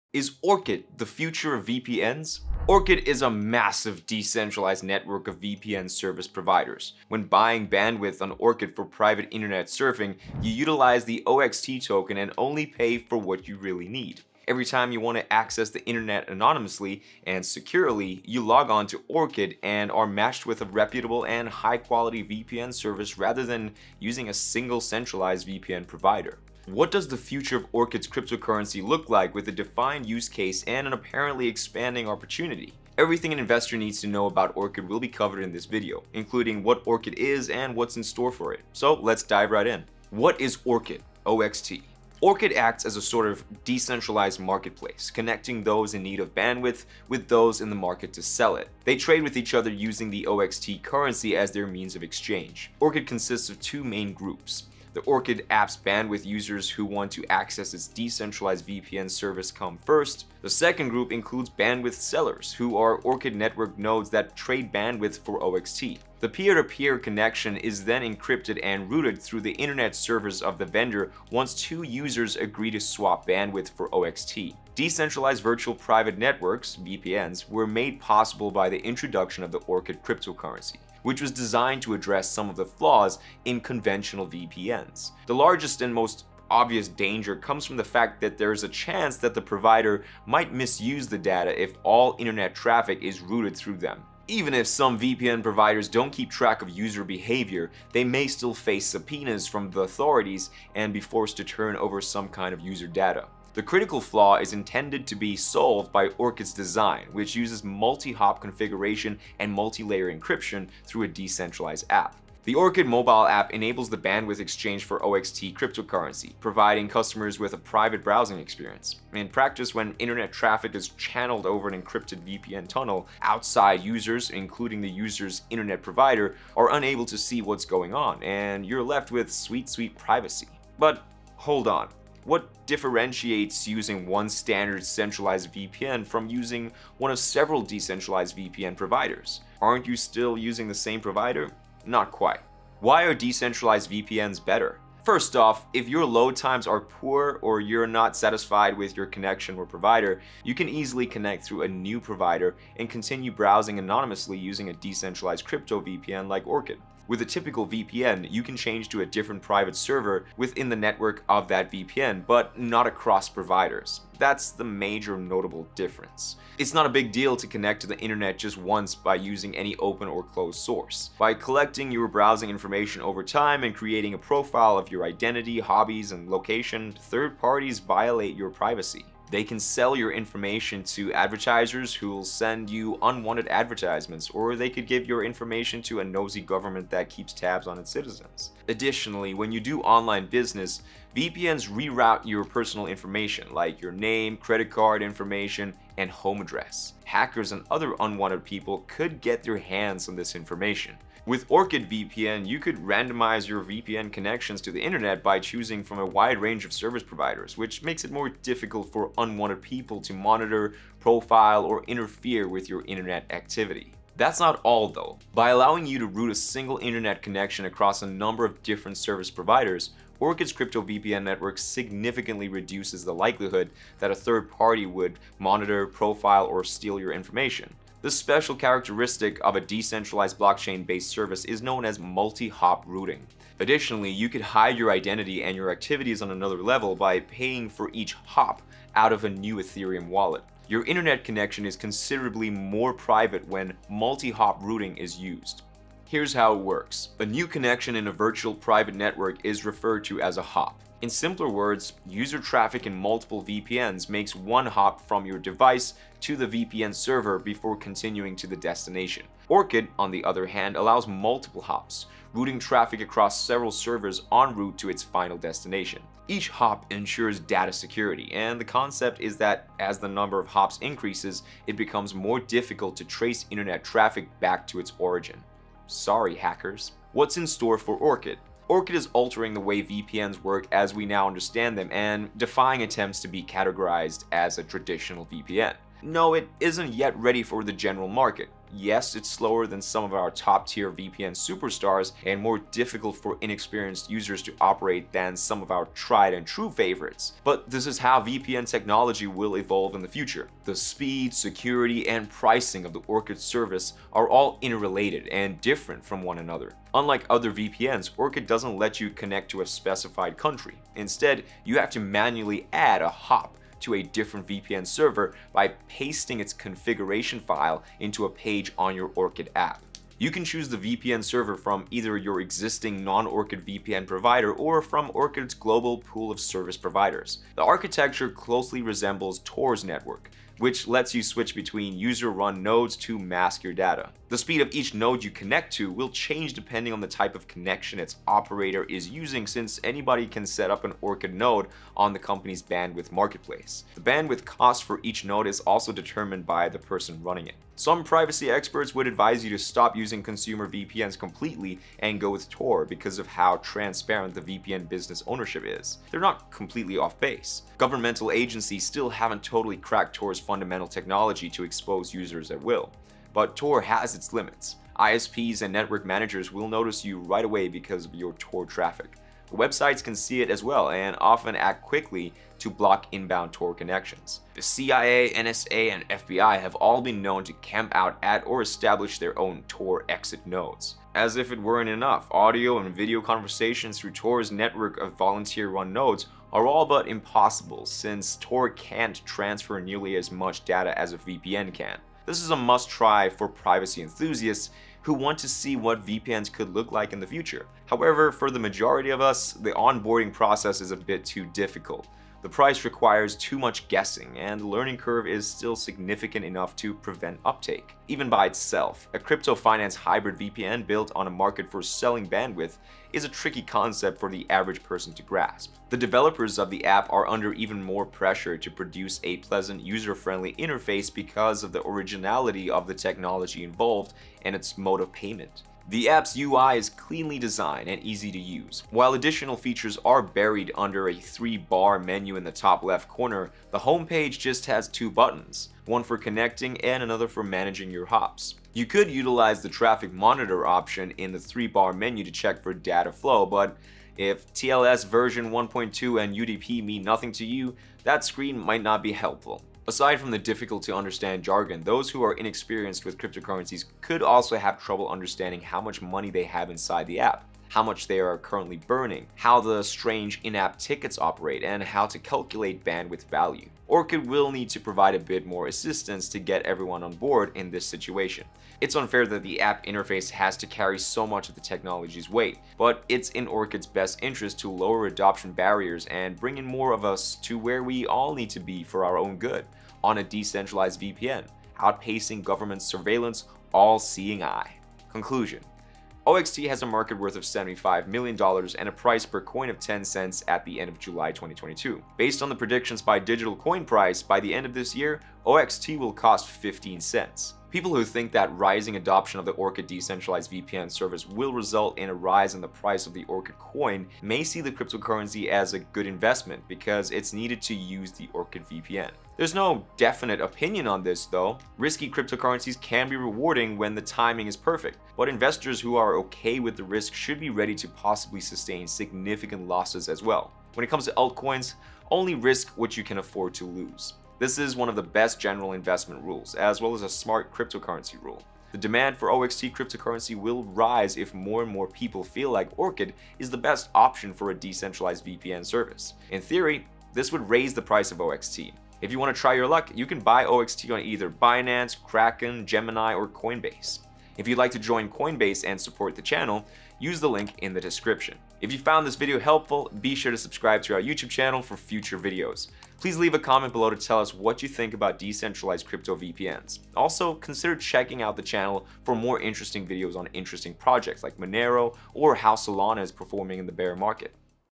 Is Orchid (OXT) The Future of VPNs?
Orchid is a massive, decentralized network of VPN service providers. When buying bandwidth on Orchid for private internet surfing, you utilize the OXT token and only pay for what you really need. Every time you want to access the internet anonymously and securely, you log on to Orchid and are matched with a reputable and high-quality VPN service rather than using a single centralized VPN provider.

What does the future of Orchid's cryptocurrency look like with a defined use case and an apparently expanding opportunity? Everything an investor needs to know about Orchid will be covered in this video, including what Orchid is and what’s in store for it.

Are decentralized VPNs the next big thing? Due to the fact that it helped spread the concept of blockchain VPNs, Orchid (OXT) is one of the most inventive blockchain ventures. Creating a decentralized virtual private network is its primary goal where you connect to different ISP's instead of different servers like in traditional VPNs.

Being decentralized, you get complete privacy and you can buy or sell bandwidth to Orchid users. Investor interest in the project has been quite high. The OXT coin hasn't had the best success in the cryptocurrency market, though. Watch our full video to learn more!